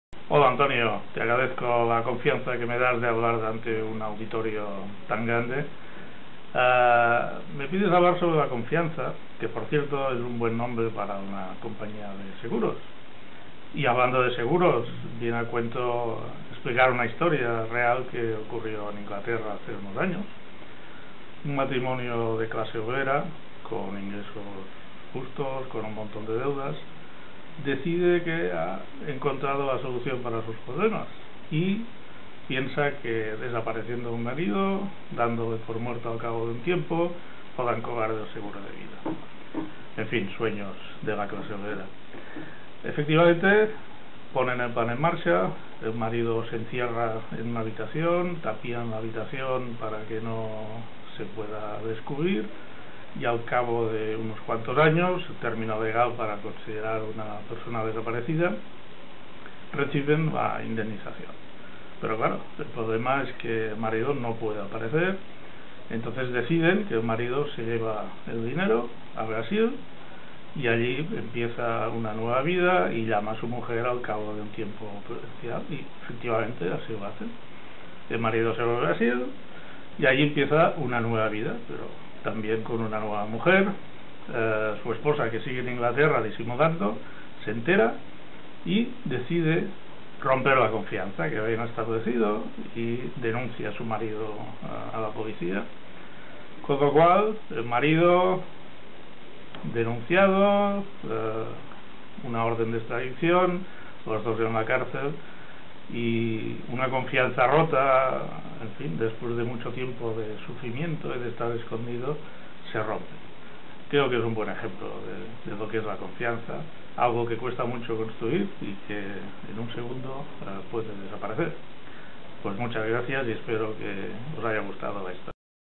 Cuestión de confianza en el matrimonio
Josep María Cos nos cuenta una historia de falta de confianza en el matrimonio